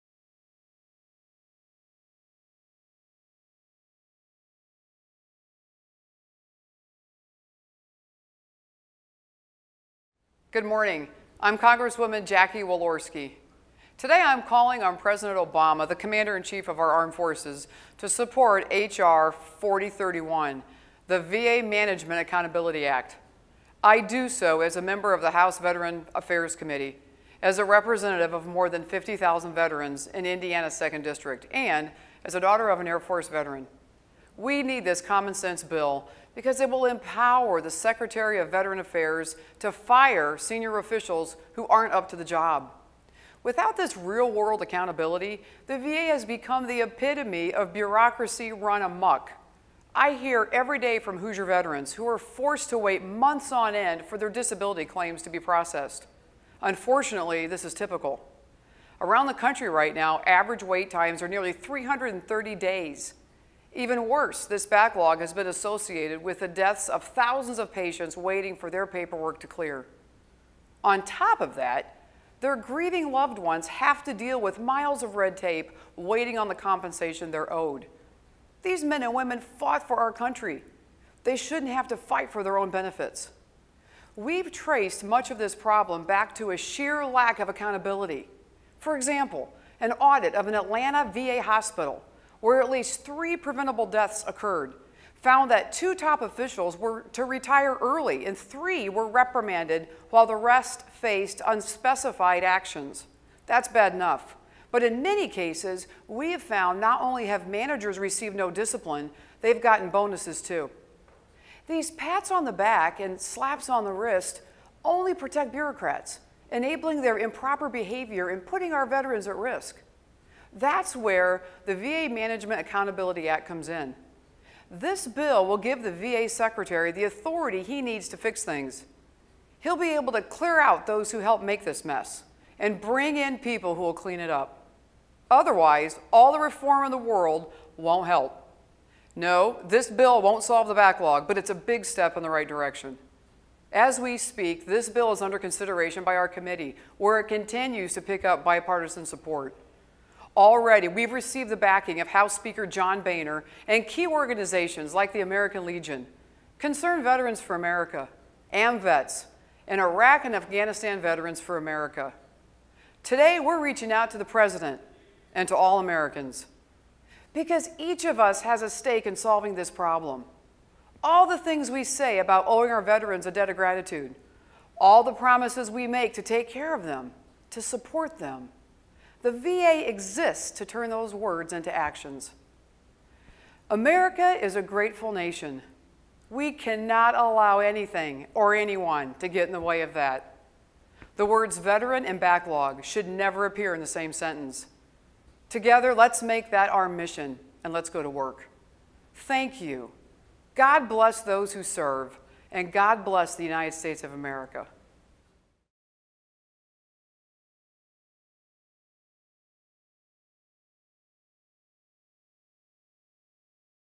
Jackie Walorski calls for Veterans Affairs reforms
Rep. Jackie Walorski, R-Ind., discusses bipartisan efforts to bring much-needed accountability to the Department of Veterans Affairs, where a claims backlog has left veterans waiting months at a time to receive their benefits.